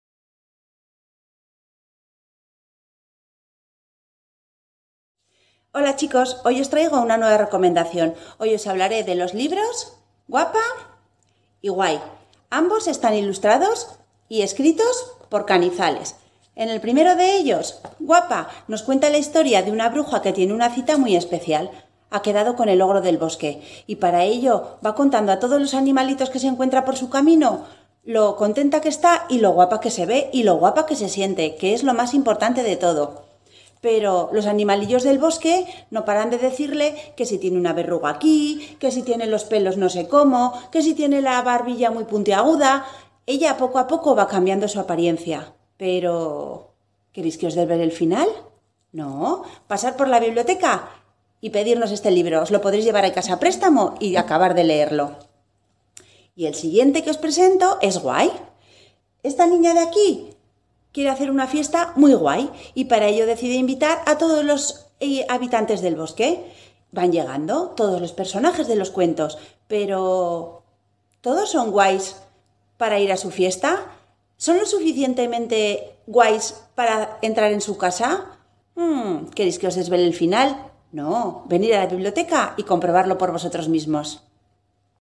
Cada día un cuento: "Guapa" y "Guay" de Canizales
Nuestras bibliotecarias te recomiendan cuentos que podrás encontrar en las bibliotecas del Ayuntamiento de Huesca

Historias fascinantes que harán que los más pequeños disfruten con la lectura.

Te recomendamos...